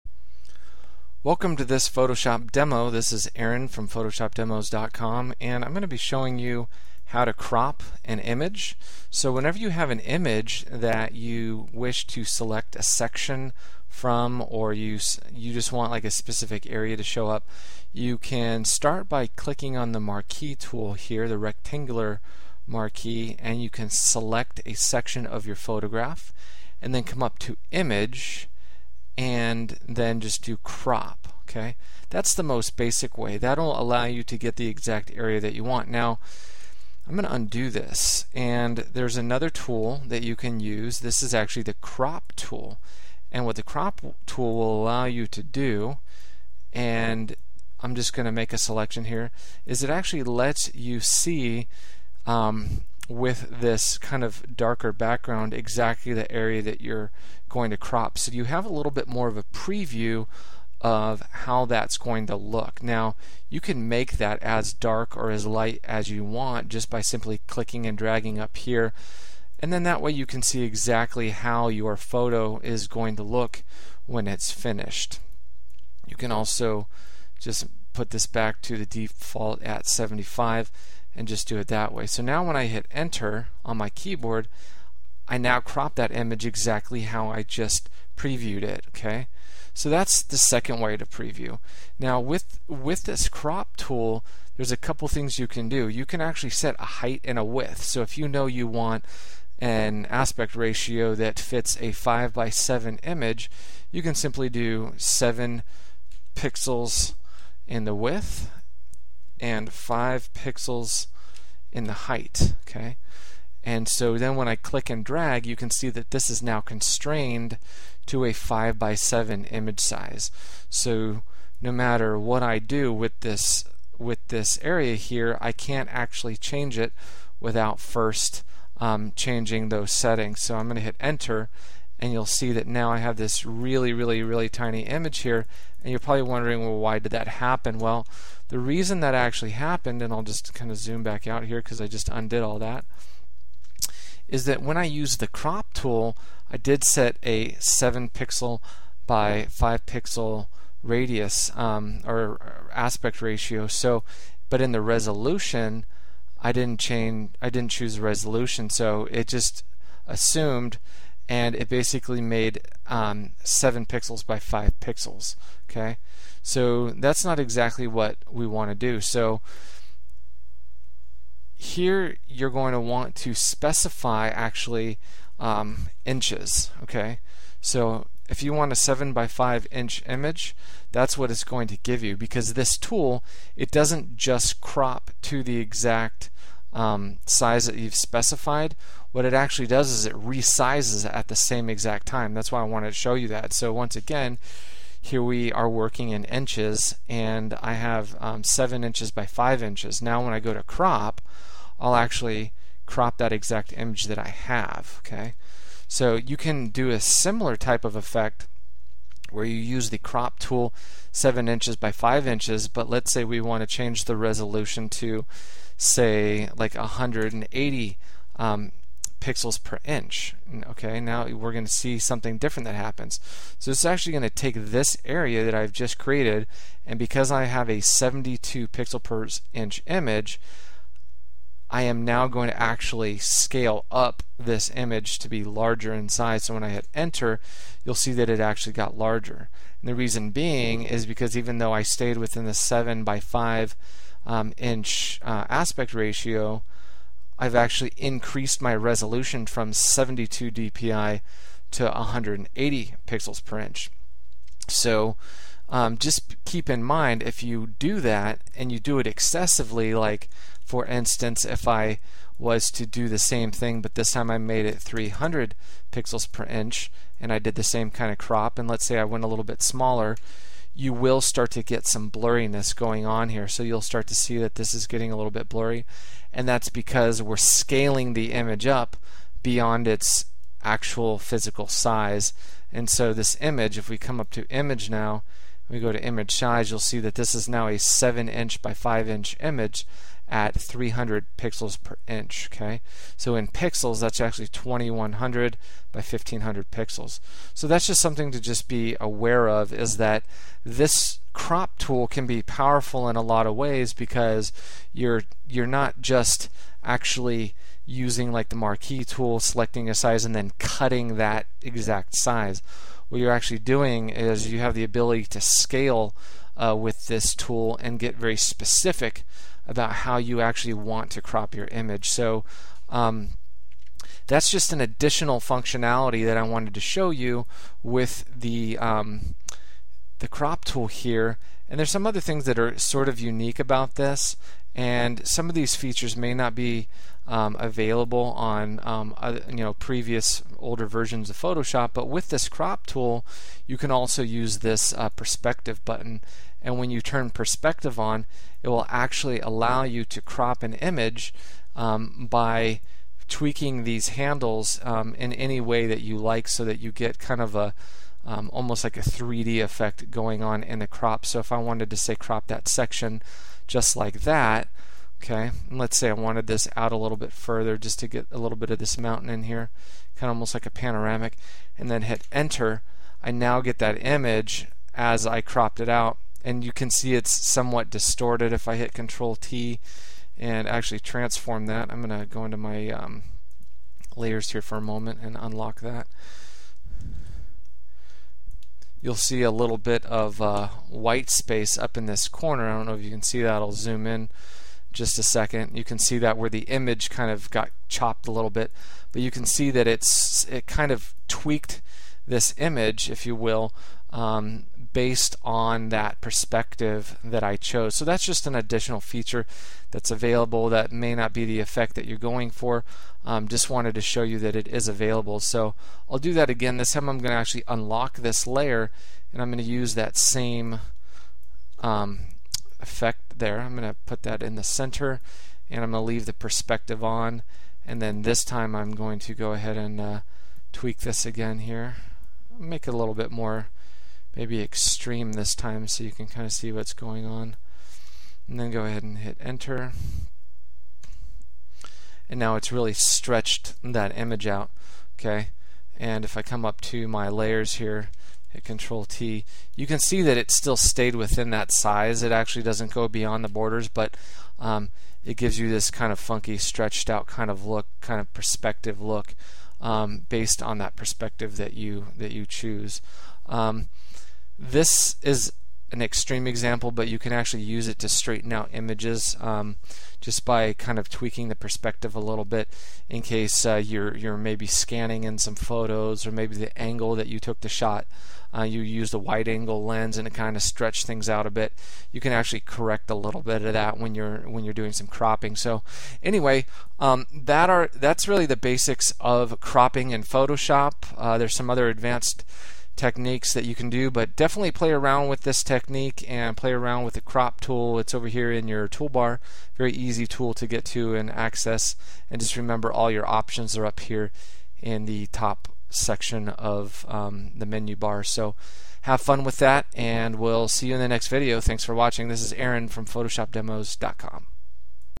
How To Crop a Photo in Photoshop
Watch step by step how to crop a photo in Photoshop. Learn how to use the crop tool and rectangle Marquee. You can re-size, scale and constrain proportions.